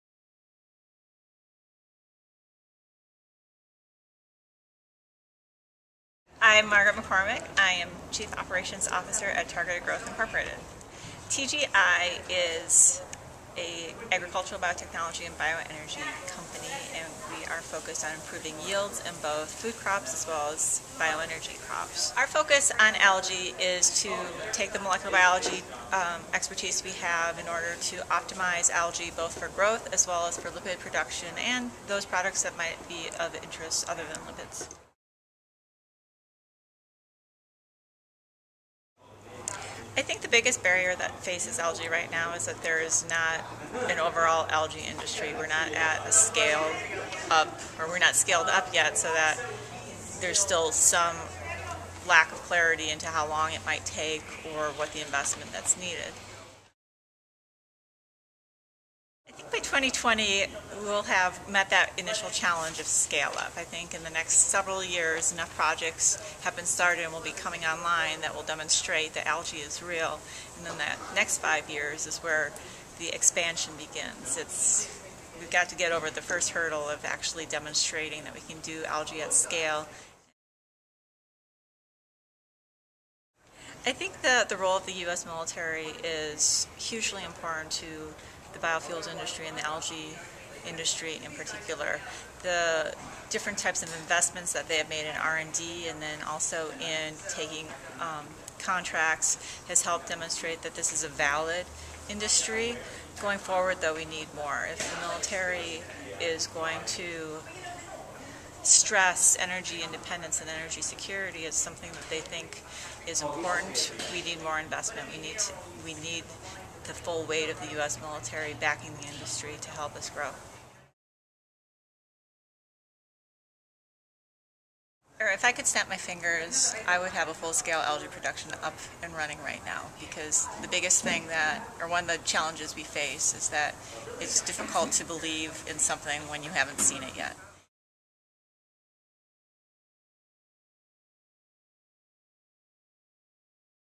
Perspectives from Leaders in the Algae Industry - Dr. Margaret McCormick, TGI
Dr. Margaret McCormick, COO of Targeted Growth, Inc, and Chairperson of the Algal Biomass Organization's Board of Directors, discusses the current and future status of the algae industry.